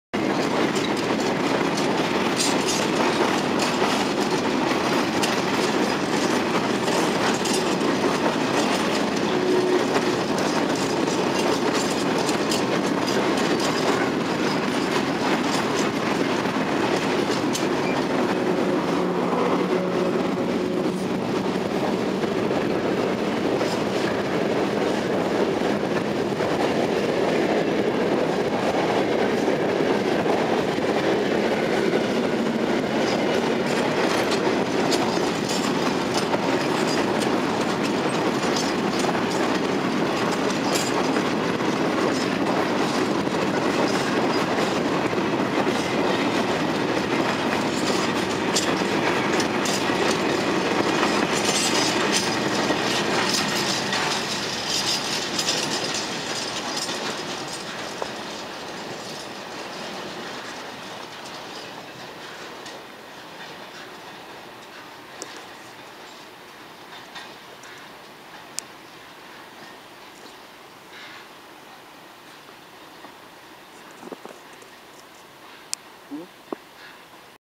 EP07 - 362 TLK "Tetmajer" & ET22 - 2017
Szlak Częstochowa Osobowa - Podg. Wyczerpy. Linia D29-61.
TLK "Tetmajer" na czele z EP07 - 362 relacji Zakopane - Gdynia Główna odjeżdza z Dworca Głównego w Częstochowie. Drugoplanowo skład wagonów, które ciągło Cargowskie ET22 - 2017.